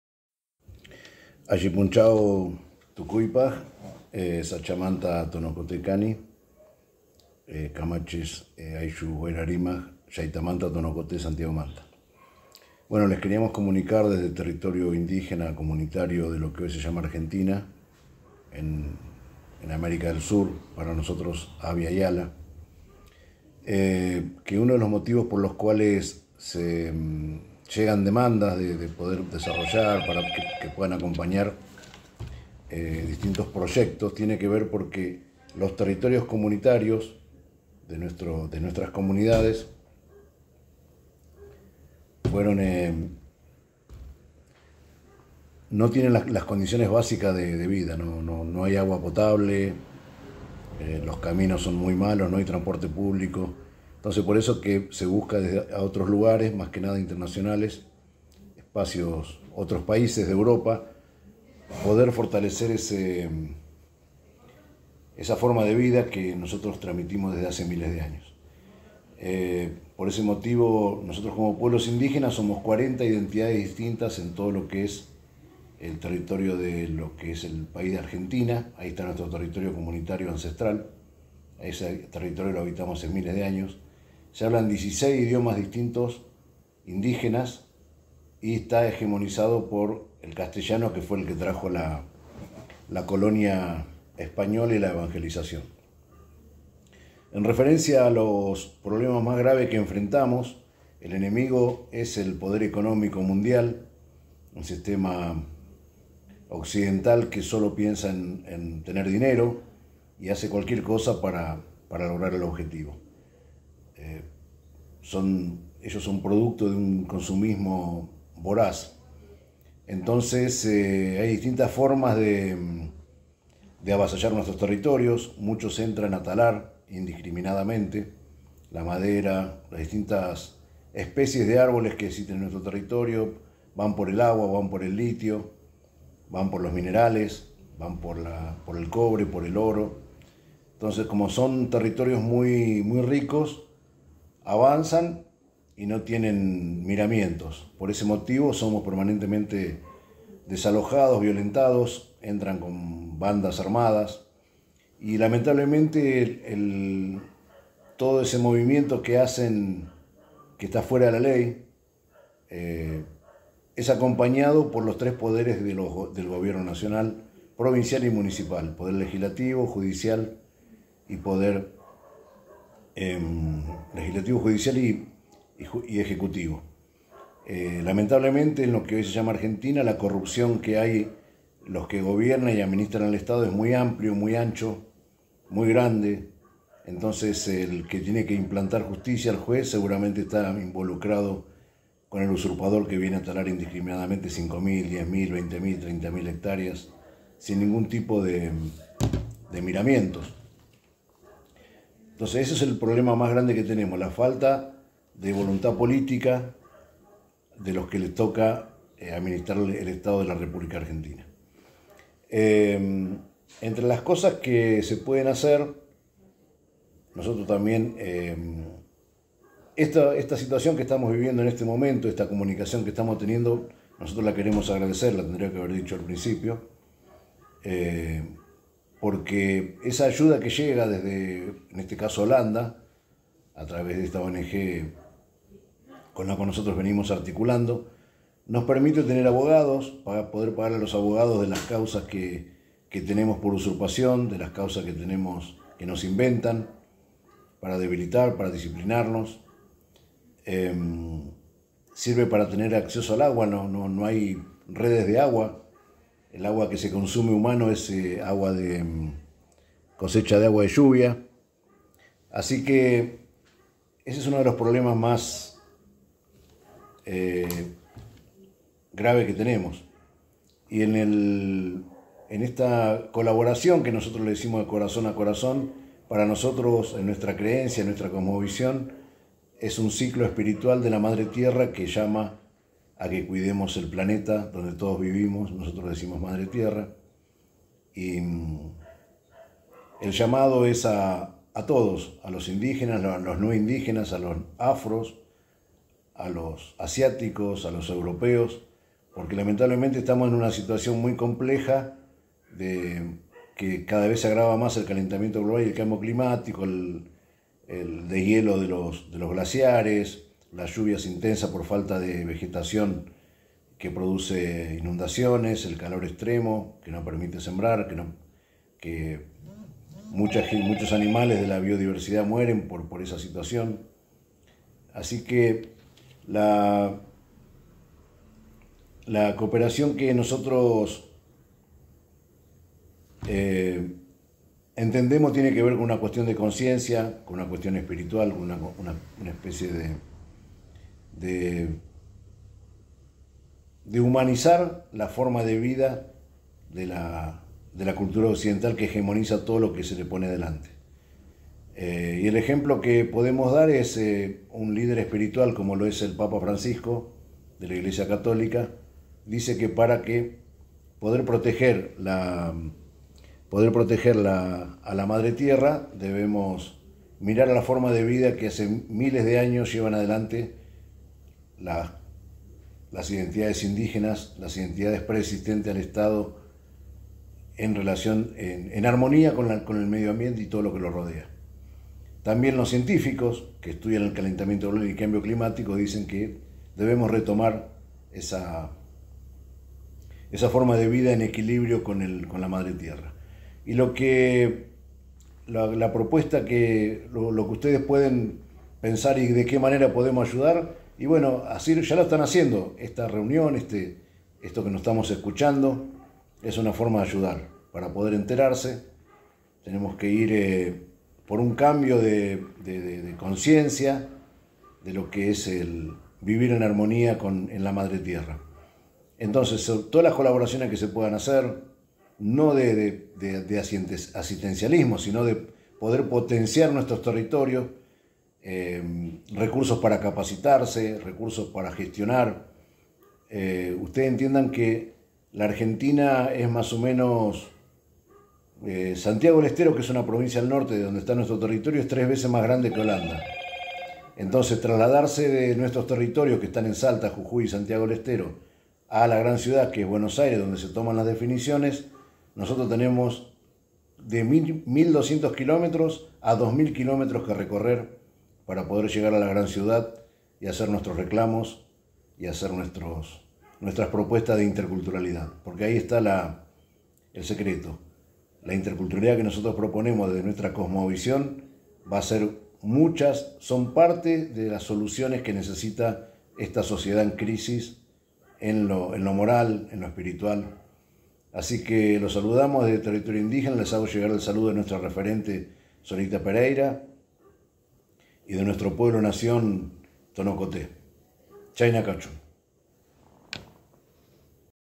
Presentatie Walter Barraza, inheemse gemeenschap Tonokoté, webinar 14 maart 2024
Walter Barraza, leider van de inheemse gemeenschap Tonokoté in Argentinië, vertelt over de gevolgen van onder andere klimaatverandering voor zijn volk.